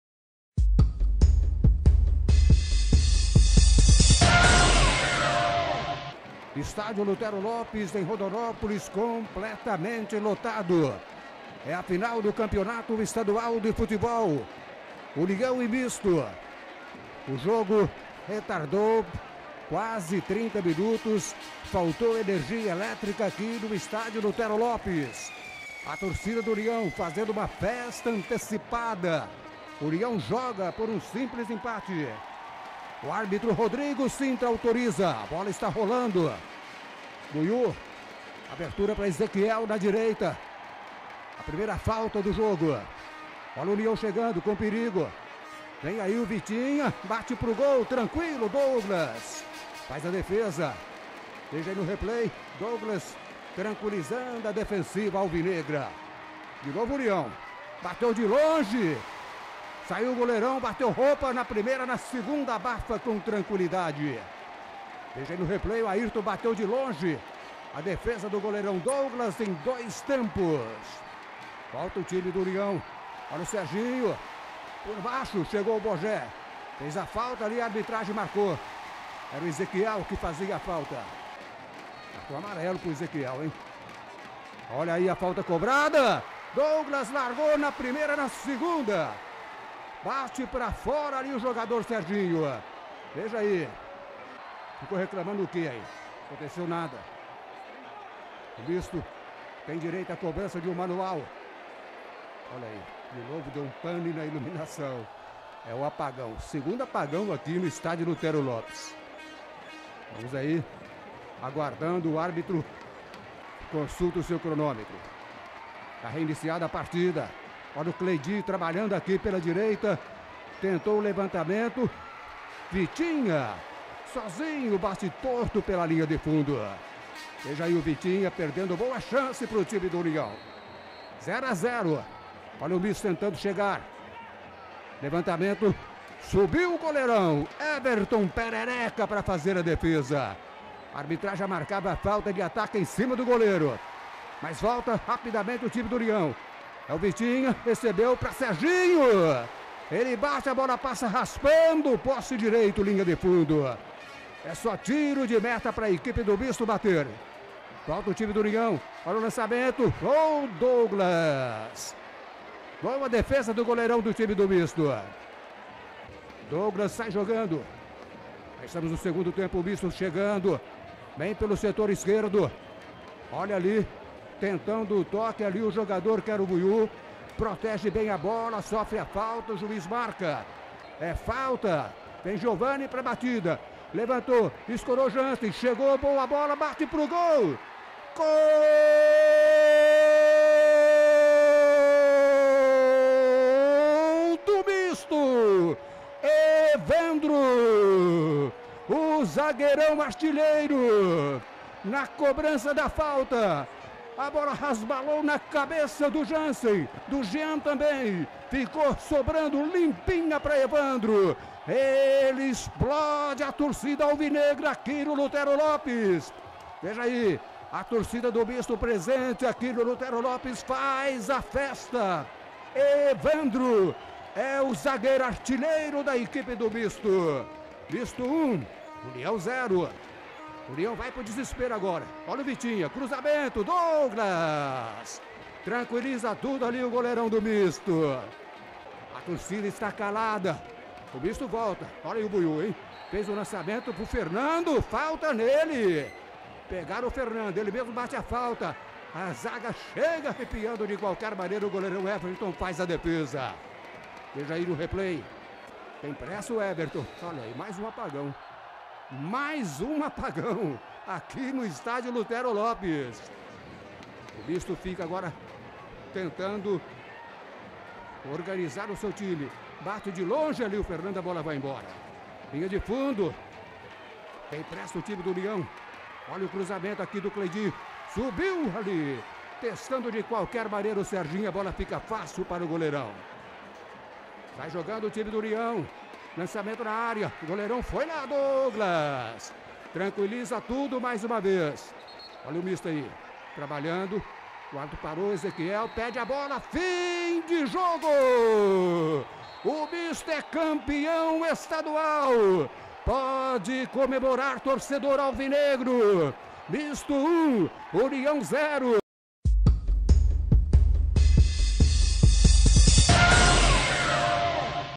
Mixto x União final Mato-grosssense 2008.wmv
MIXTO E UNIÃO DECISÃO DO CAMPEONATO MATO-GROSSENSE DE 2008
O MIXTO VENCEU EM RONDONÓPOLIS POR 1 X 0
NARRAÇÃO DE RUBENS NEVES TV RECORD CUIABÁ